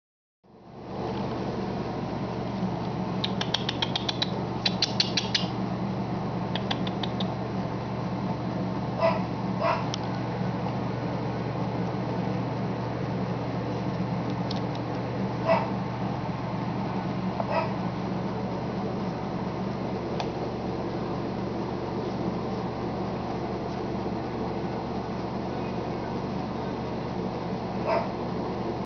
6 week old Yorkie Pups
2 seperate litters - 22 hours age difference! Little one is 1/4 the weight of the big one!  Amzing!